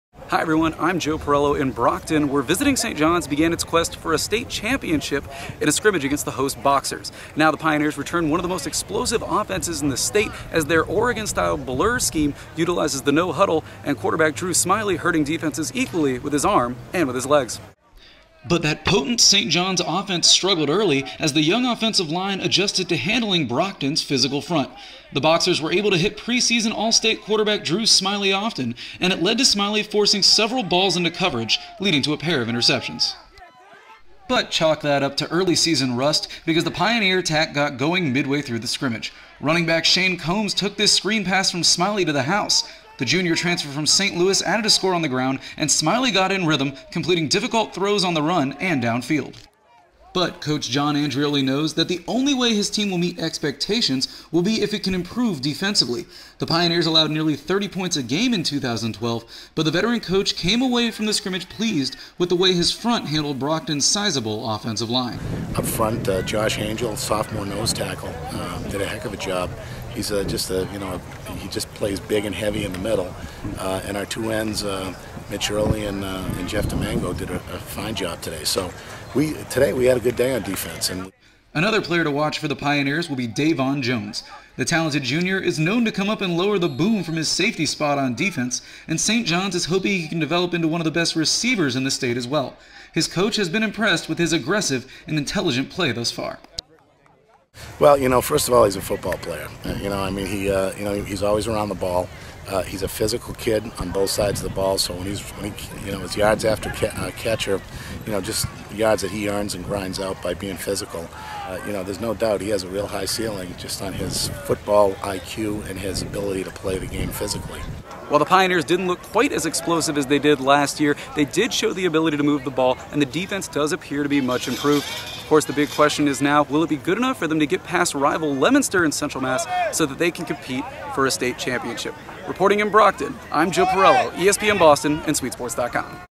St. John's (Shrewsbury) Football Preview 2013
Joe Parello takes in the Pioneers' scrimmage at Brockton, and catches up with head coach John Andreoli afterward.